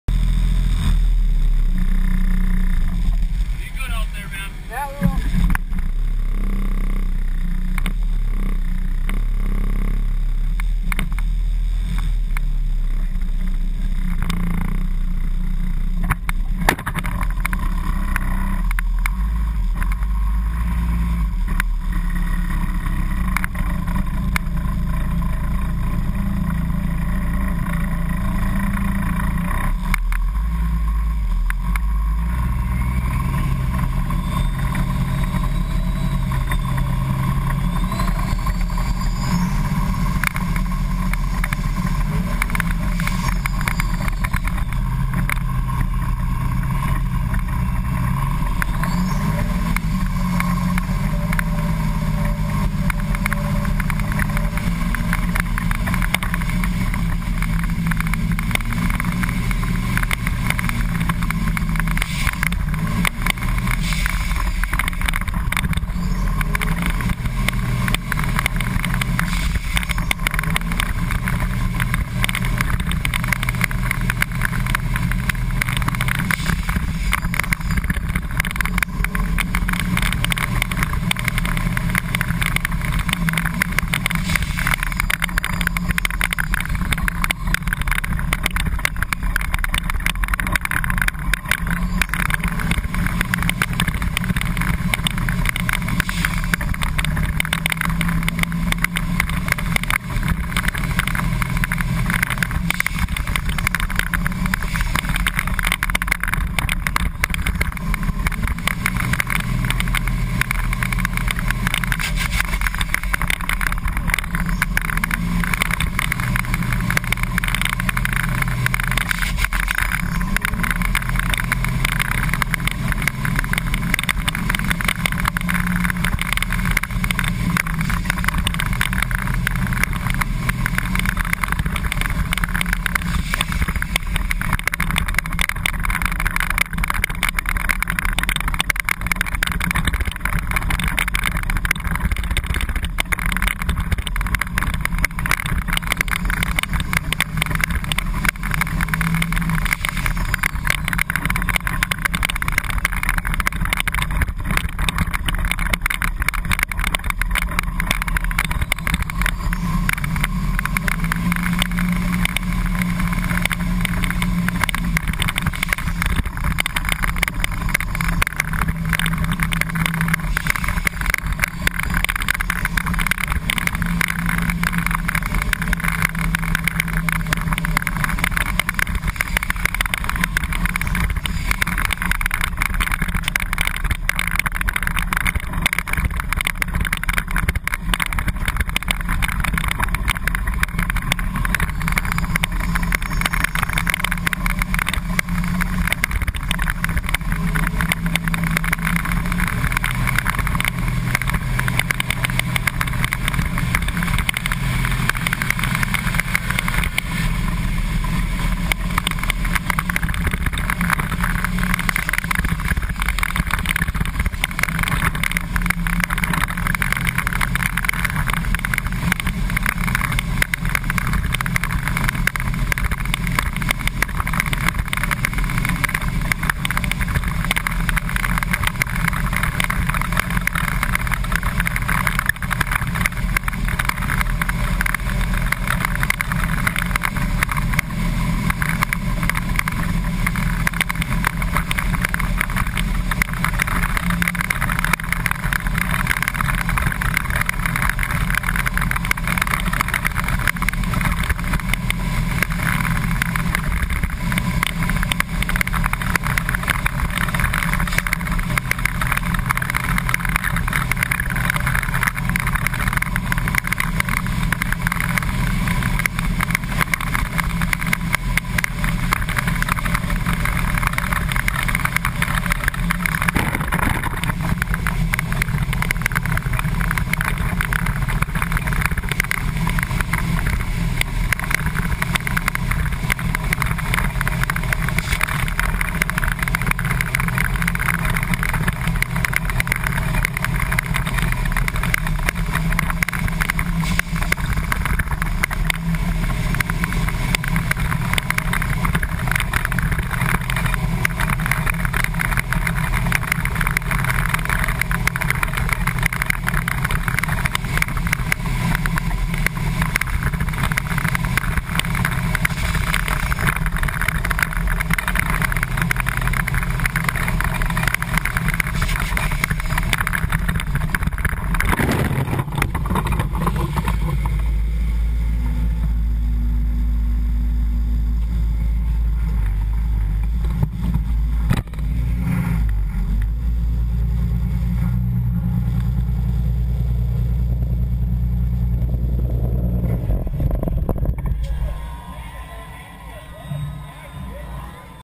School Bus Race Crash GoPro Dash cam Footage
Ogilvie Raceway hosted the 3rd annual Battle of the buses,  four buses battled it out, Tom Kohl in the Onamia flat nose  bus crashed.  Watch the footage to see the flames!